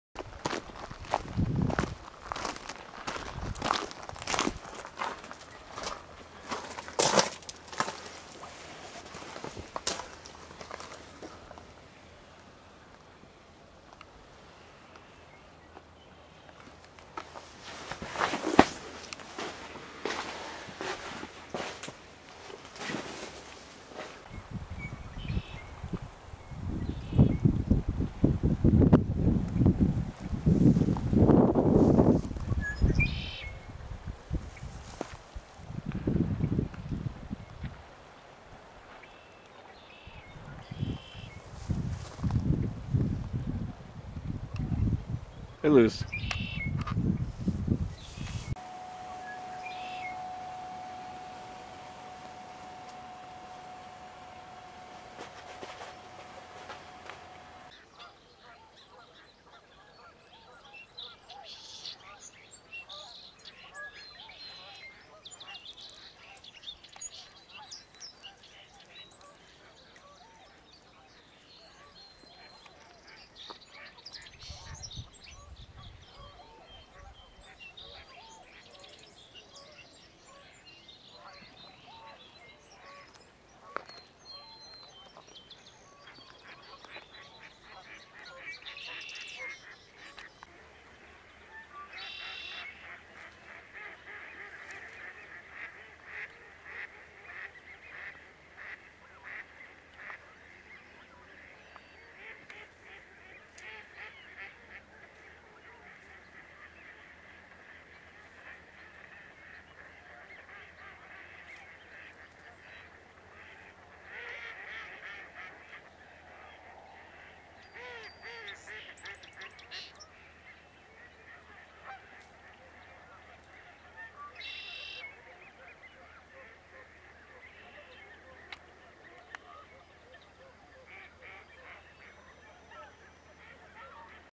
Kodiak Canvas Cabin Tent Camping
I love camping with this tent.  I'll save the nylon for backpacking!  The location is Bruneau Pool off CJ Strike Reservoir (Snake river), Idaho.